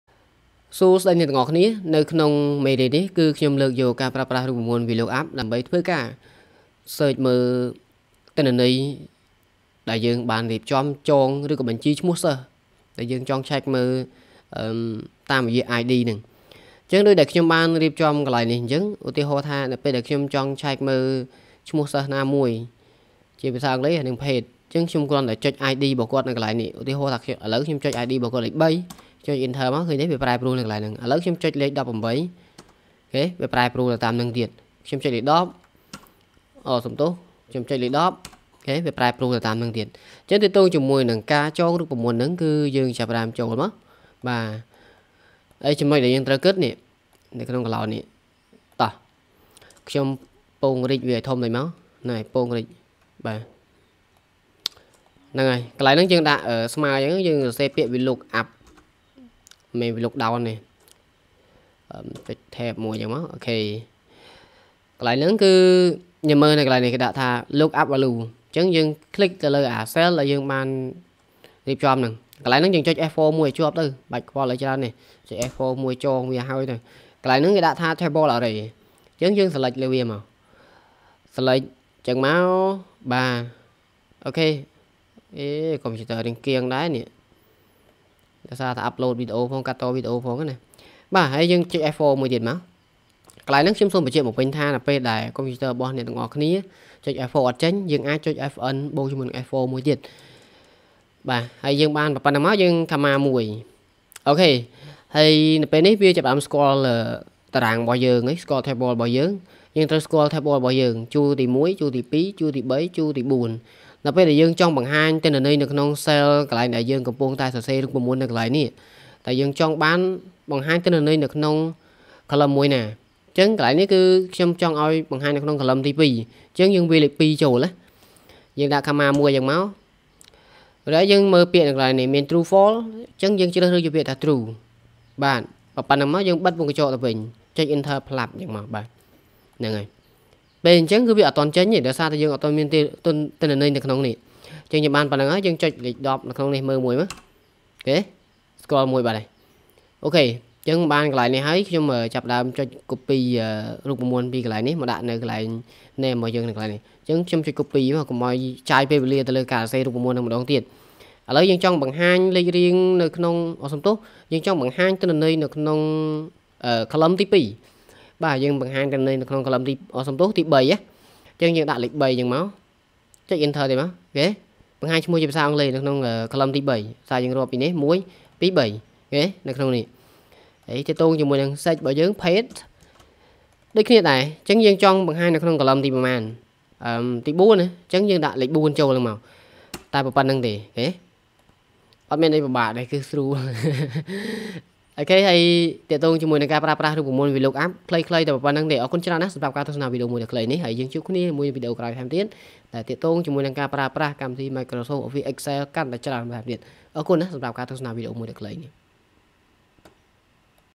#17 ការប្រើប្រាស់រូបមន្ត Vlookup Microsoft Office Excel 2019
What is Microsoft Excel Vlookup?
VLOOKUP stands for 'Vertical Lookup'. It is a function that makes Excel search for a certain value in a column (the so called 'table array'), in order to return a value from a different column in the same row.

Excel VLOOKUP Function
1 value - The value to look for in the first column of a table.
2 table - The table from which to retrieve a value.
3 col_index - The column in the table from which to retrieve a value.
4 range_lookup - [optional] TRUE = approximate match (default). FALSE = exact match.